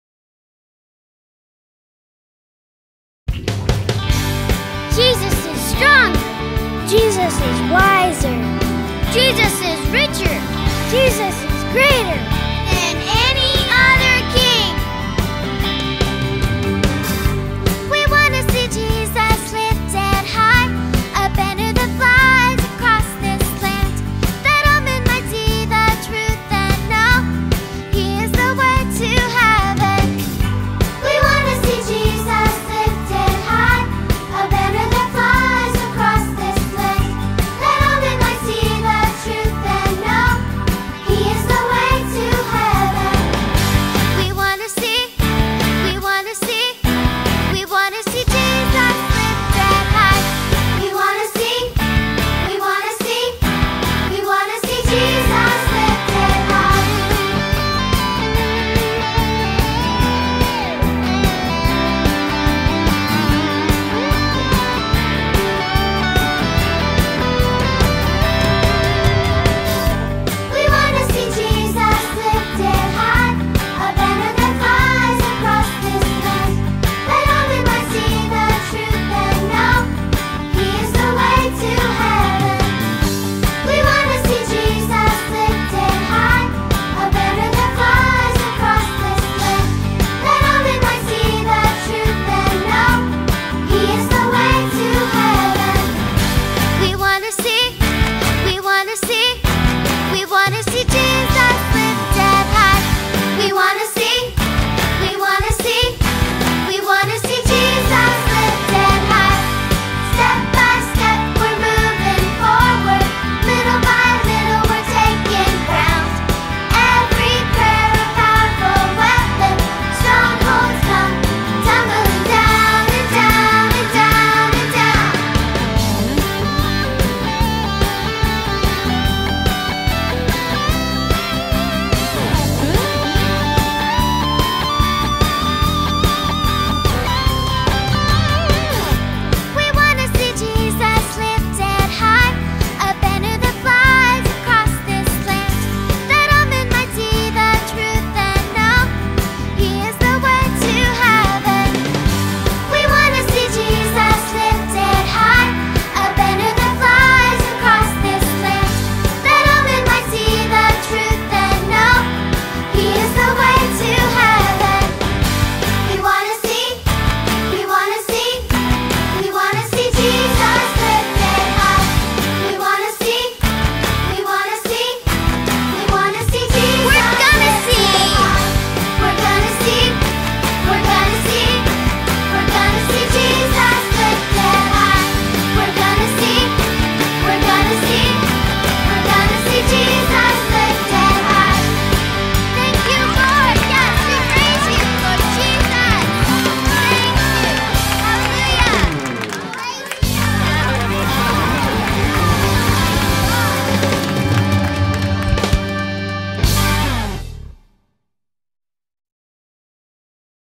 3B Little Disciples!
This is the video I made for our Palisades Summer Christian School final day presentations that played during our ROCKING performance : )
These are my wonderful kids from our 3B class! I miss them all very much!